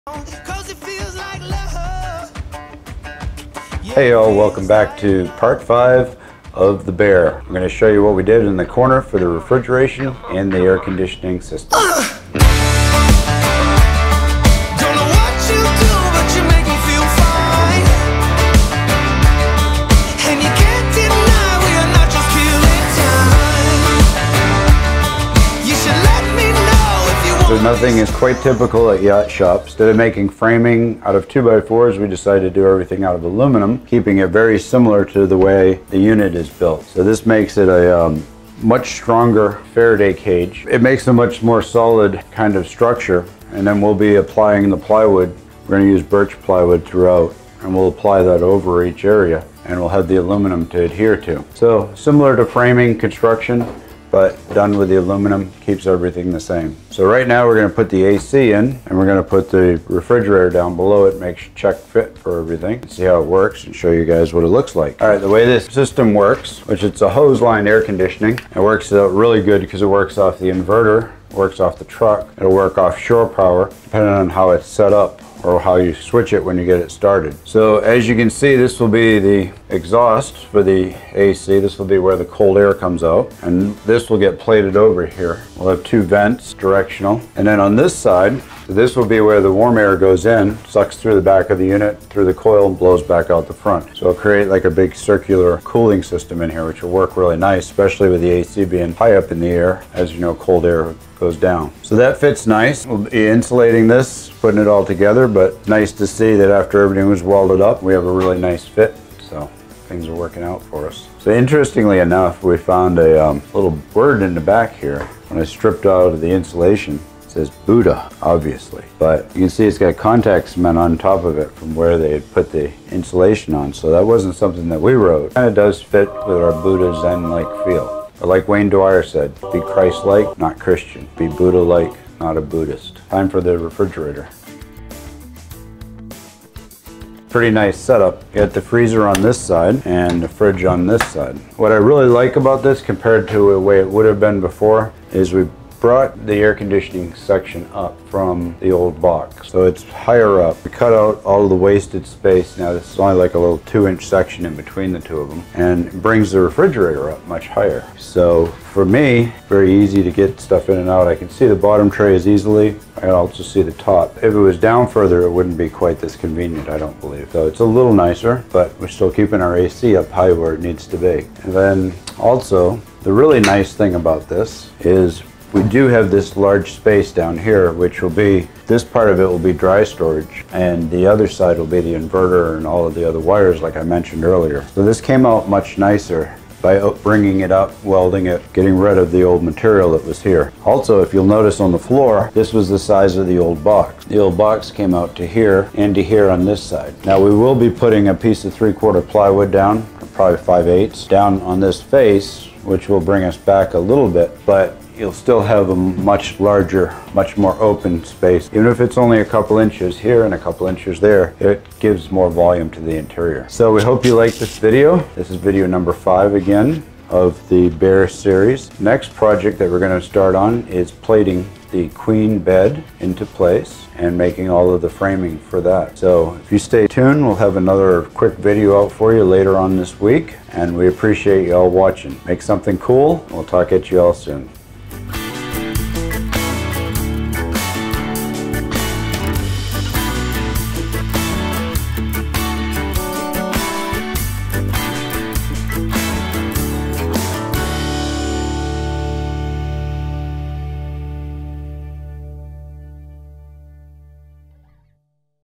Ambulance to RV/Part 5 - Yacht Shop Creations "The Bear" Project
Good afternoon! In this weeks video Jimbo talks about the newest additions made for the Refrigeration/AC Corner Unit!

Also Jim we showcase the work Jimbo and the crew did on the  latest Yacht!

Thanks again for all the views and comments!